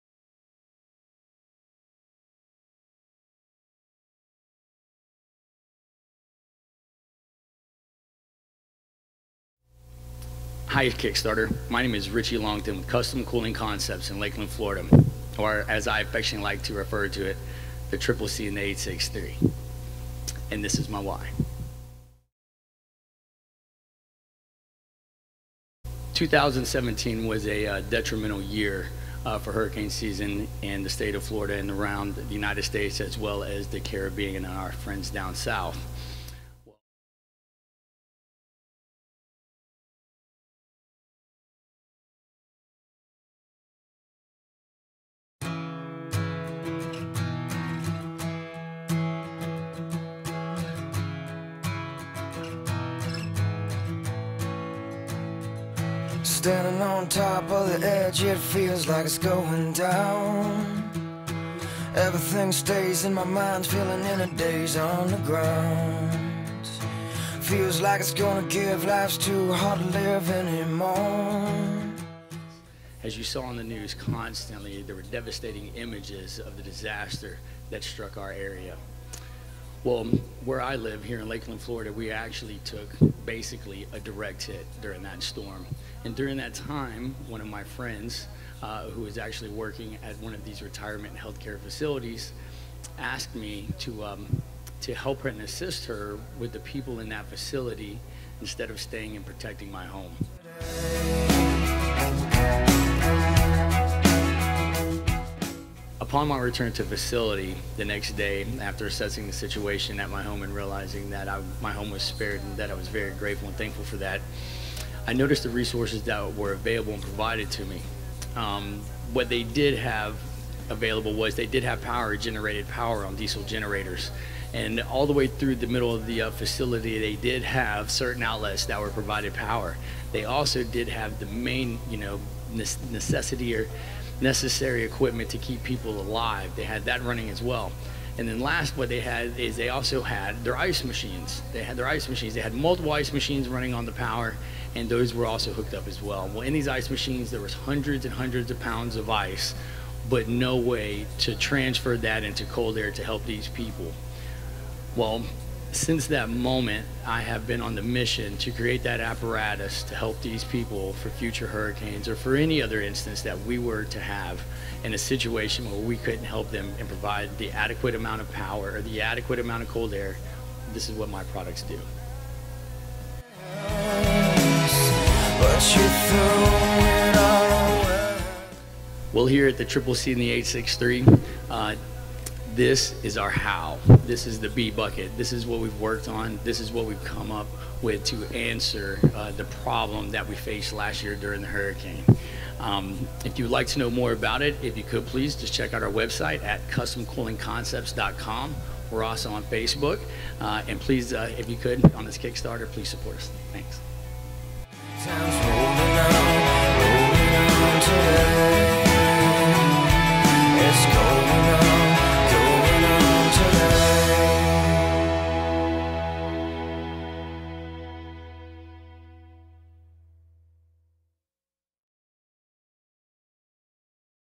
Richy Longtin, CCC Kickstarter Video
The creation of Custom Cooling Concepts (CCC).